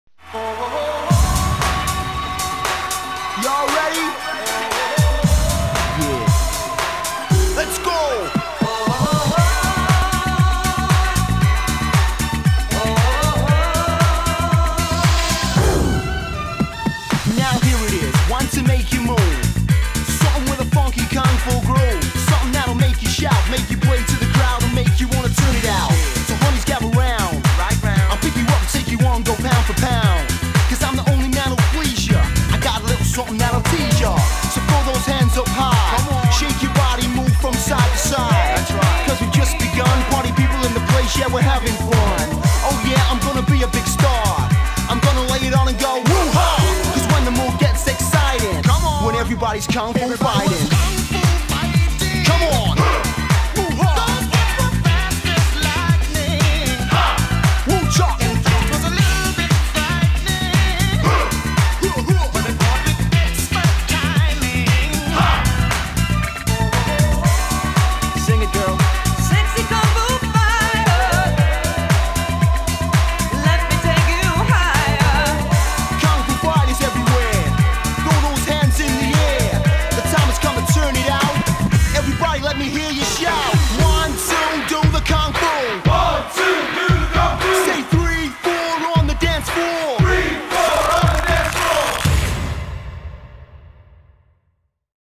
iClone Animation, Arbalest Dance Take 2
Updated, longer, more camera control, effects, prop swaps